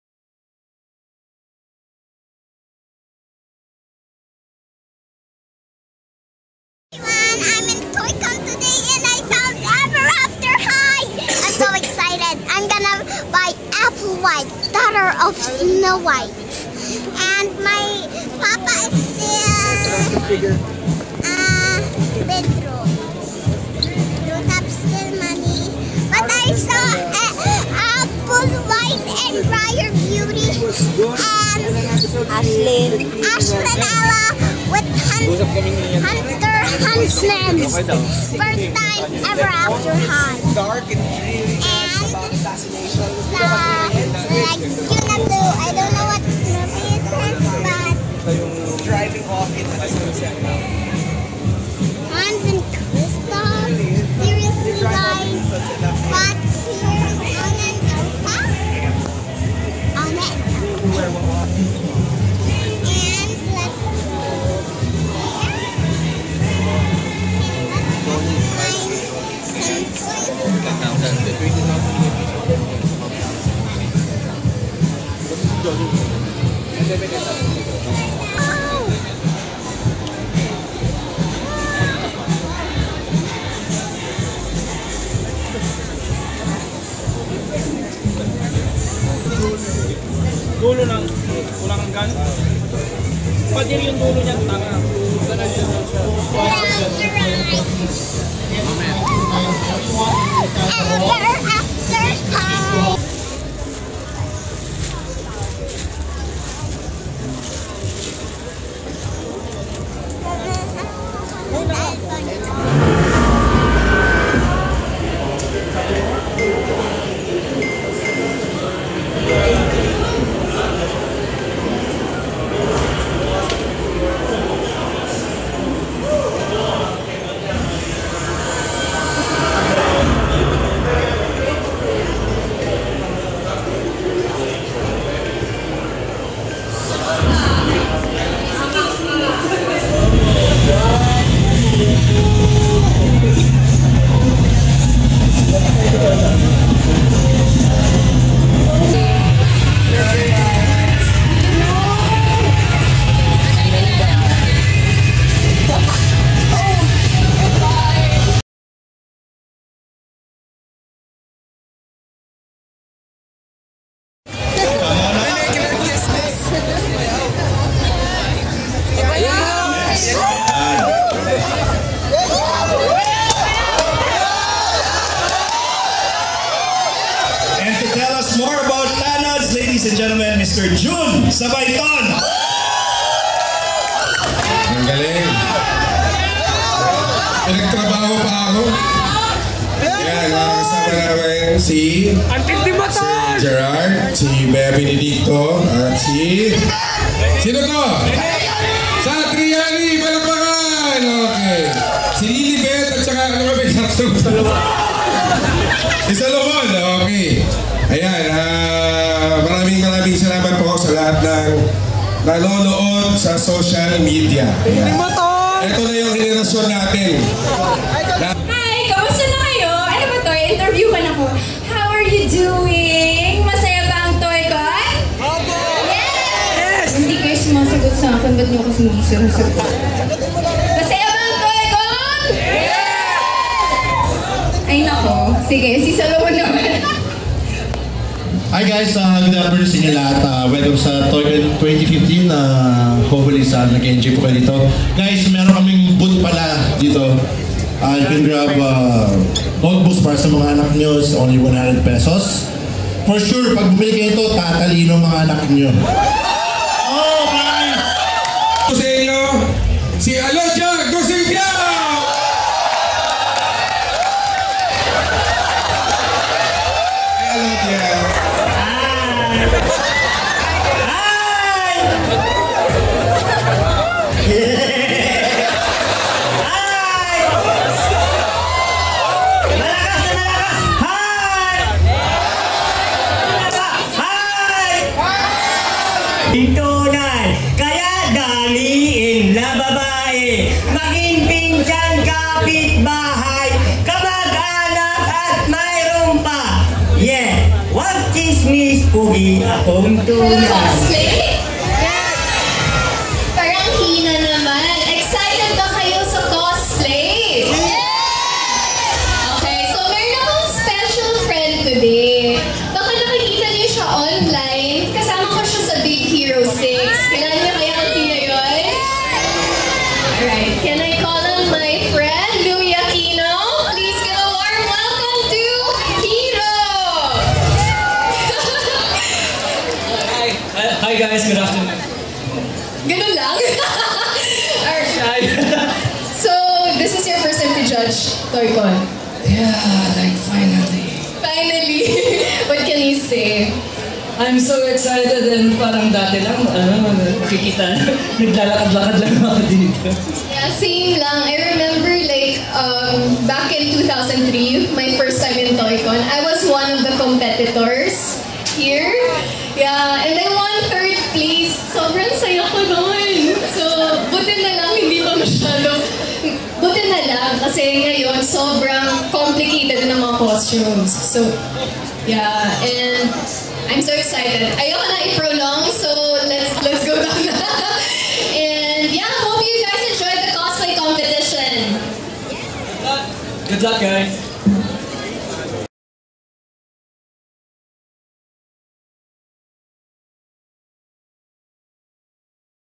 TOYCON 2015 Conblog
Philippine Toys, Hobbies and Collectibles Convention 2015
June 19,20,21 2015
@ SM Megamaall Trade 1-3 5th floor

Those of us who grew up in the pre-social media era or even the pre-electronic gaming age remember a time when “Play” was associated with the outdoors. We fondly remember halcyon days of setting up toy soldiers in the hilly terrain of our own backyard or lining up our Star Wars action figures to go into battle with our He-Man toys in the wonderland of our childhood bedroom.

he Philippine TOYCON, Asia’s biggest and longest running convention for toys, anime, comics, cosplay, gaming and anything and everything related to hobbies and collections is now on its 14th year of thrilling fans and aficionados alike. It is, simply put, the biggest pop culture event in the country.